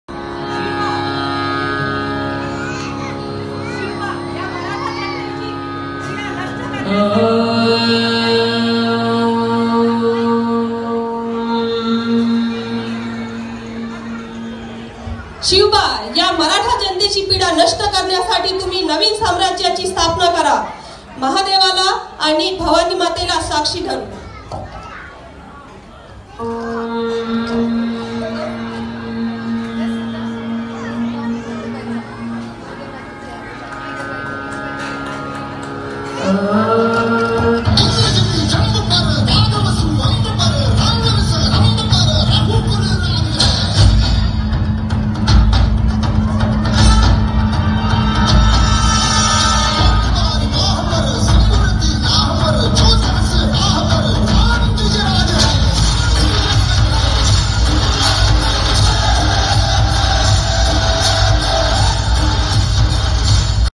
A little Shivaji Maharaj
Kid performing the story of Shivaji Maharaj with her mother on "Darlings_of_Nagpur" event...
Emprize Event and Entertainment